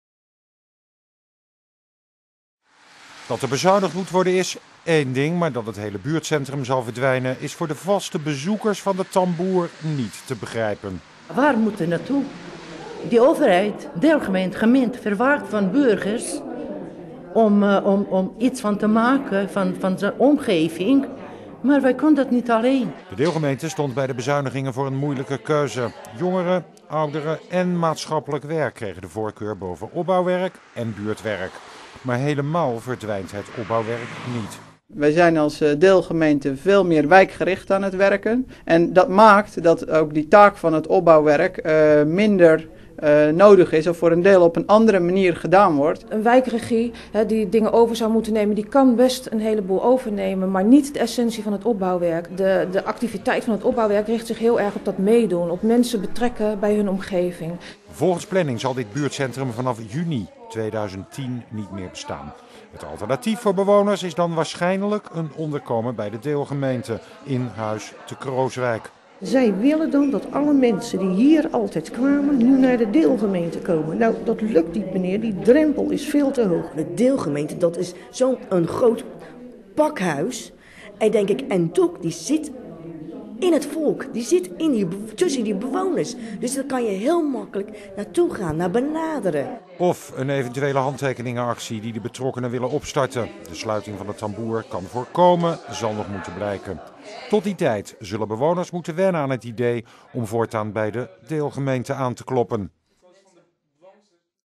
Kralingen-Crooswijk bezuinigt fors op opbouwwerk
De deelgemeente Kralingen-Crooswijk gaat 450.000 euro bezuinigen op het opbouwwerk in de wijk. Dat betekent dat er 6 tot 8 banen gaan verdwijnen. Daarnaast wordt het wijkcentrum de Tamboer gesloten.